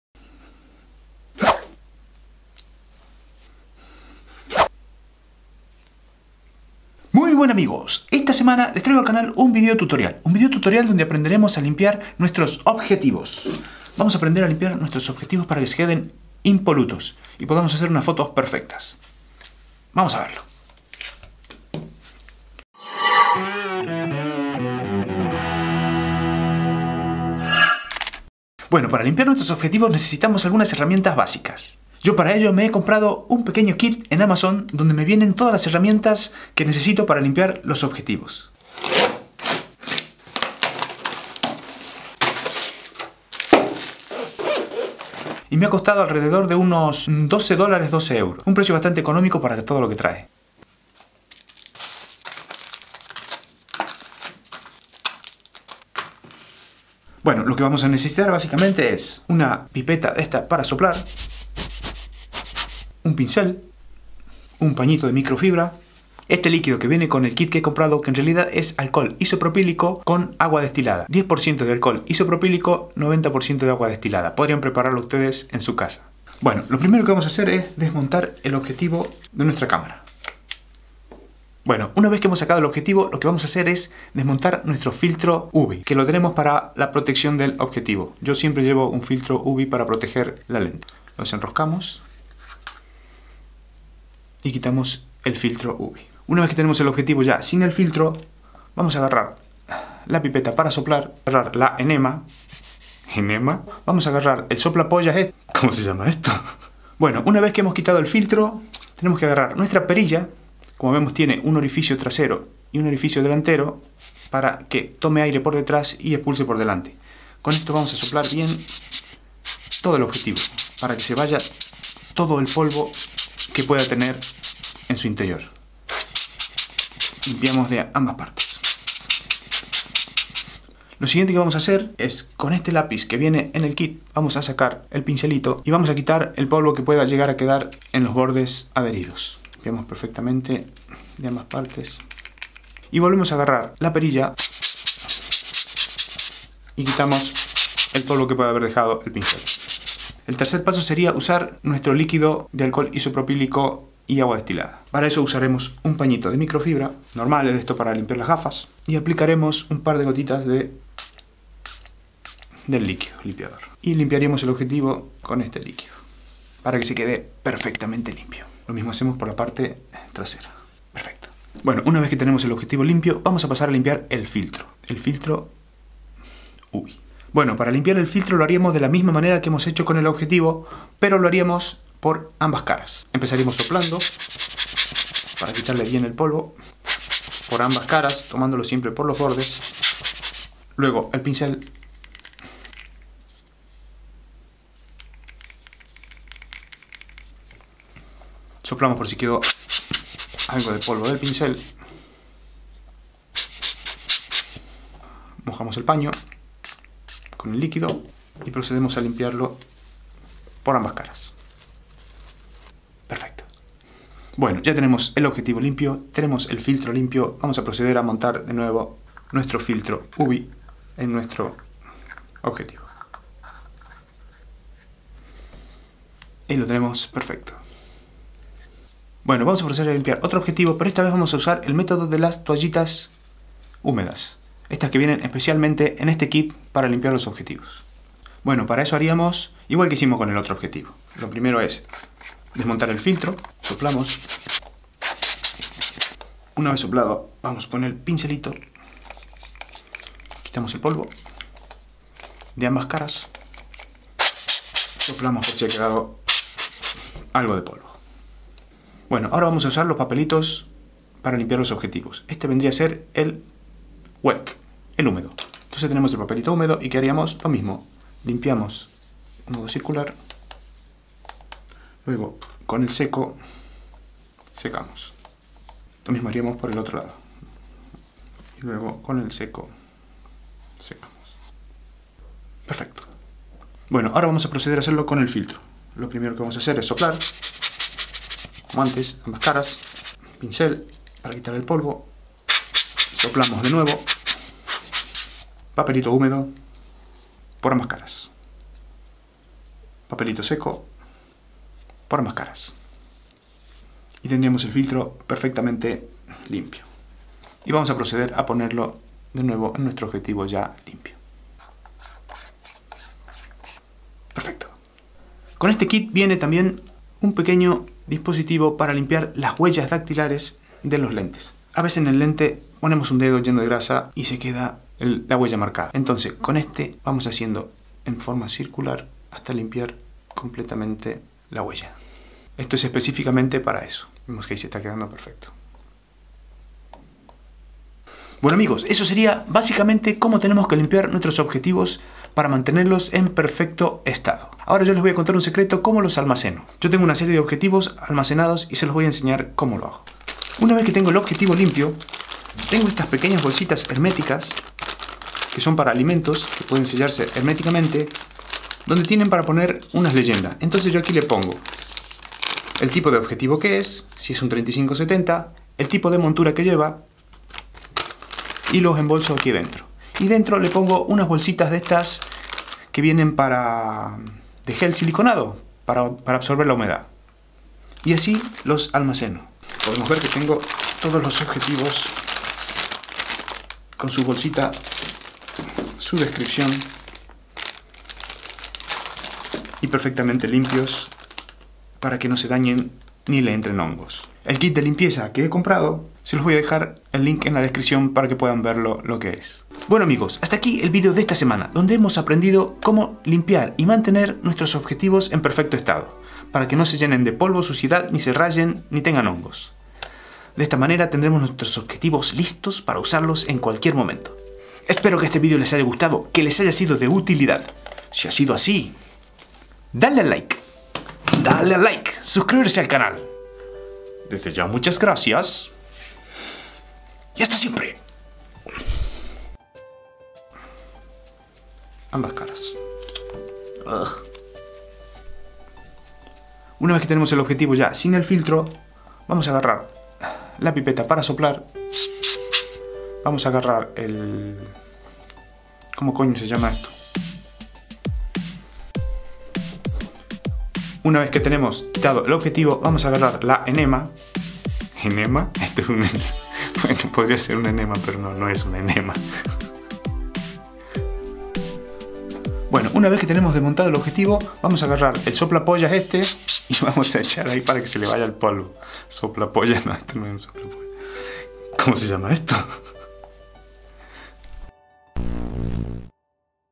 Como limpiar objetivos cámara reflex
muy buenas

hoy estoy festejando mi cumpleaños y como no podía ser de otra manera compartiendo un video con ustedes.

esta vez les traigo un video tutorial donde aprenderemos a limpiar y cuidar adecuadamente nuestros objetivos fotográficos.

Link kit de limpieza de camaras:

Link asociado a Amazon, al comprar por este link puedes apoyar al canal, sin afectar el costo para ti.

indice

00:00 Inicio

00:22 Intro
00:29 Que necesitamos para limpiar los objetivos.
00:28 Umboxing kit de limpieza
00:57 Detallamos lo indispensable para limpiar nuestro objetivo
01:20 Decribimos paso a paso el proceso de limpieza
03:48 Limpiamos un objetivo con pañitos para limpieza de objetivos.
05:15 Como limpiar las huellas digitales de los objetivos.
05:45 Como almacenar los objetivos para mantenerlos perfectos.
06:42 Final y despedida
07:10 Tomas Falsas

Equipo de grabación:
Canon 70D
Canon 7D

Gracias! por comentar, subscribirse y darle a like :) Gracias!